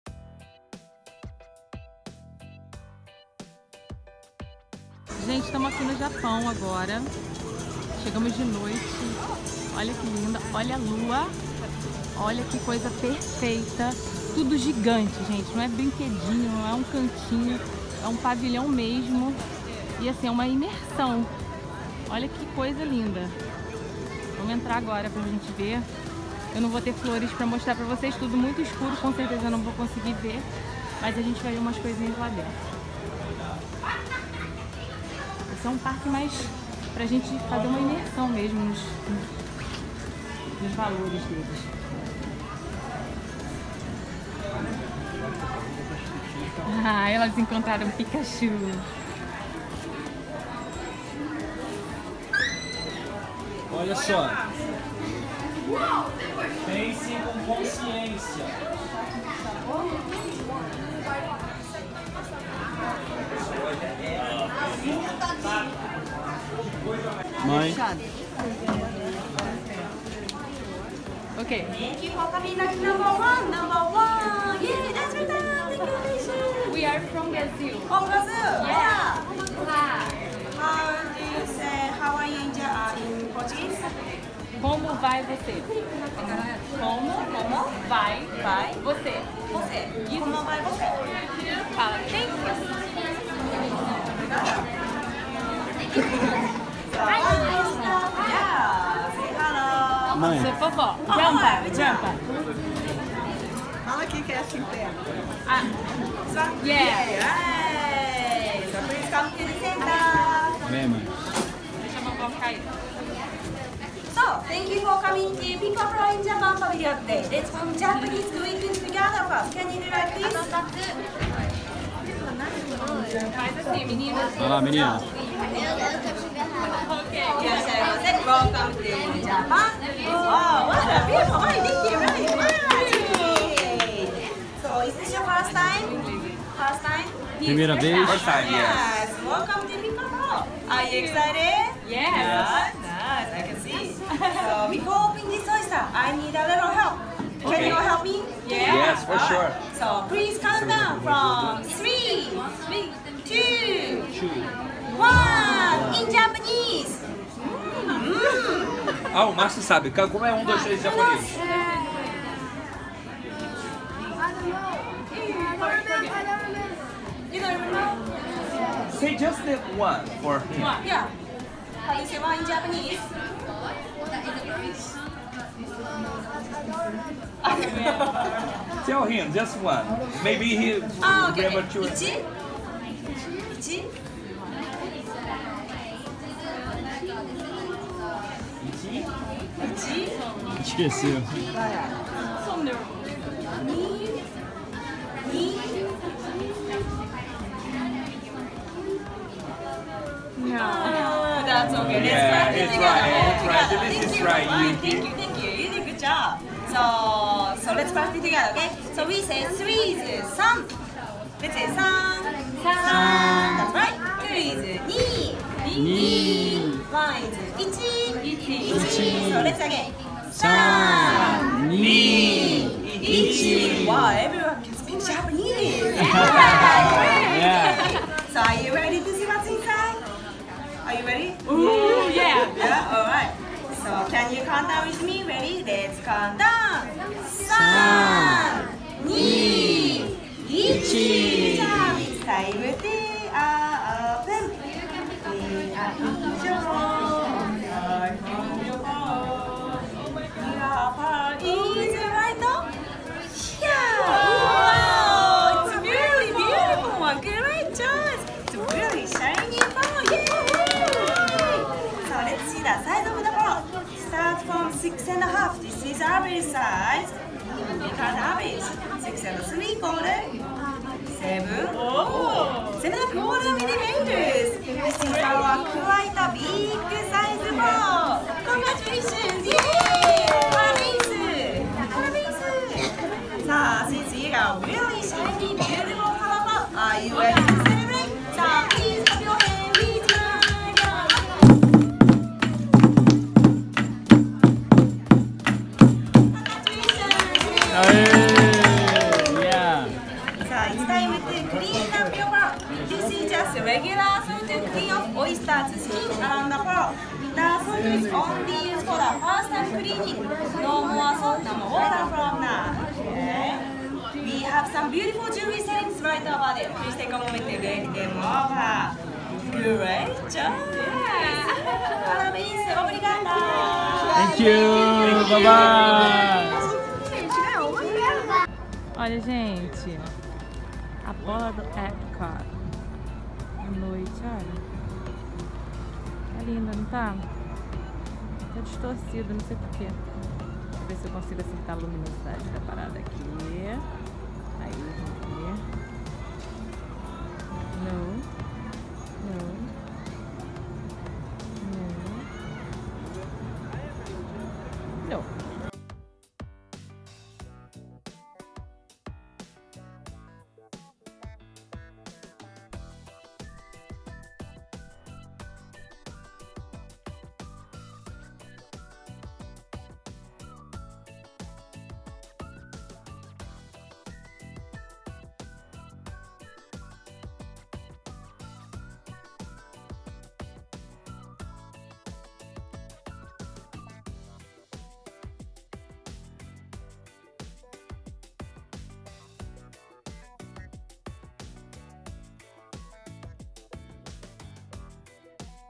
Conhecendo o Japão na Disney Epcot
Um pedacinho do Japão no Epcot Disney. E as Marias se divertiram! Vem com a gente 🇯🇵